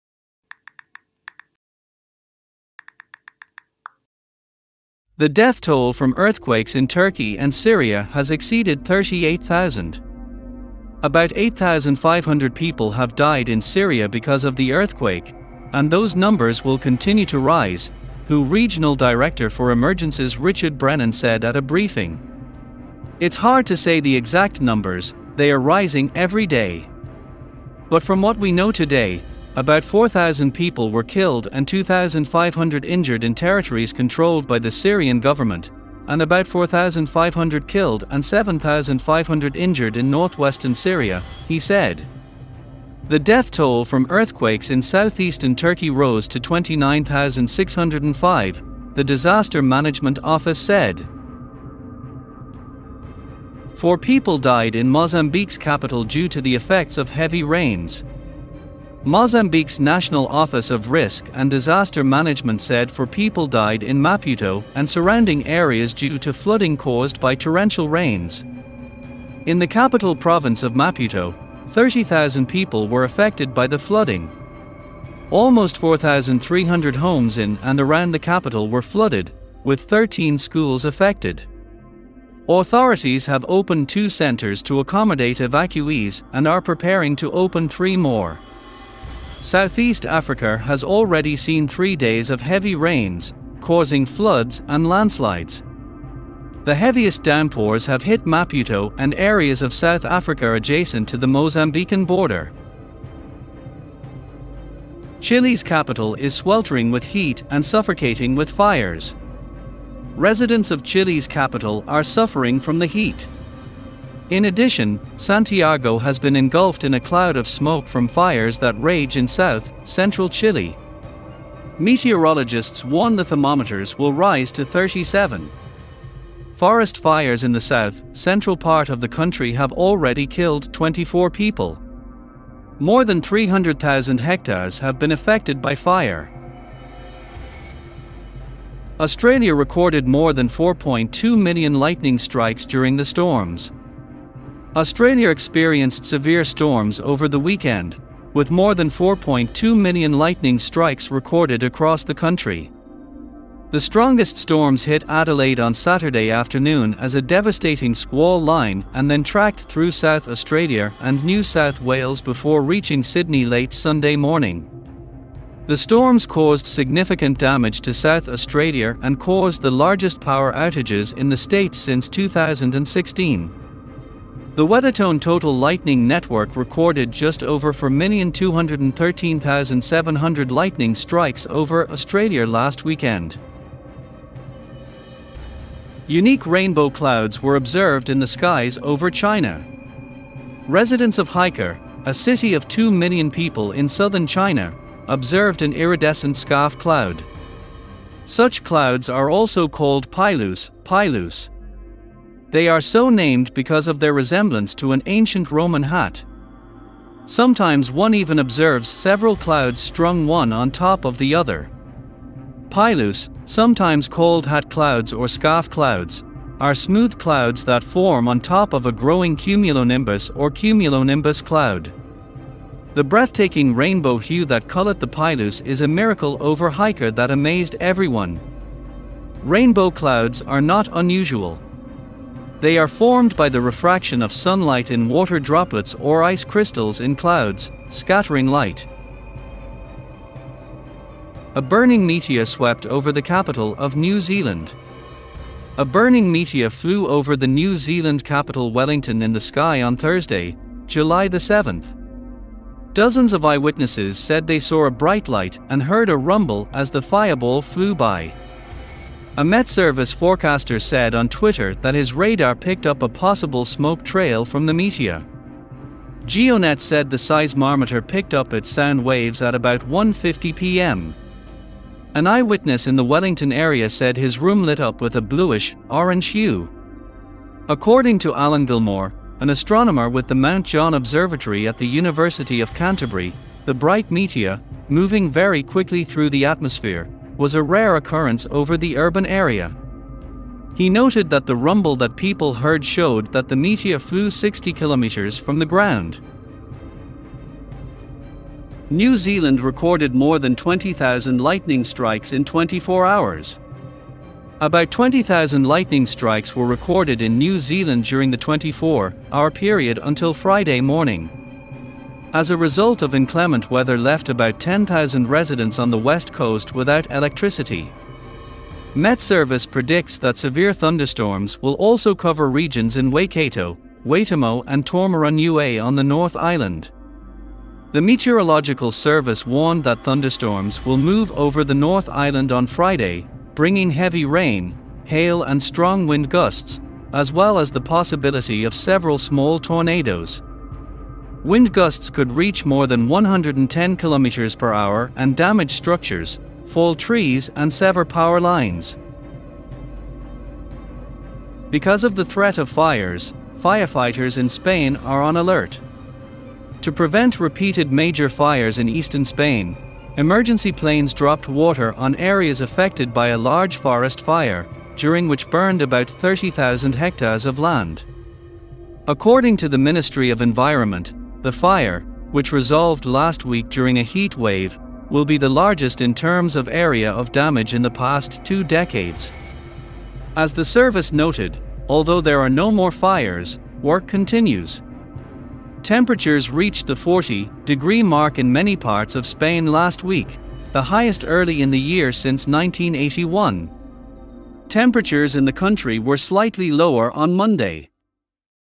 News Today 13.02.2023 - Emergency, Cataclysms
News Today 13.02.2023 - Emergency, Cataclysms, Events of the Day.
Torrential rains in Mozambique, Earthquake in Turkey and Syria, Forest fires in Chile, Massive lightning strikes in Australia, Unique rainbow clouds in China, Meteorite in New Zealand, Fire threat in Spain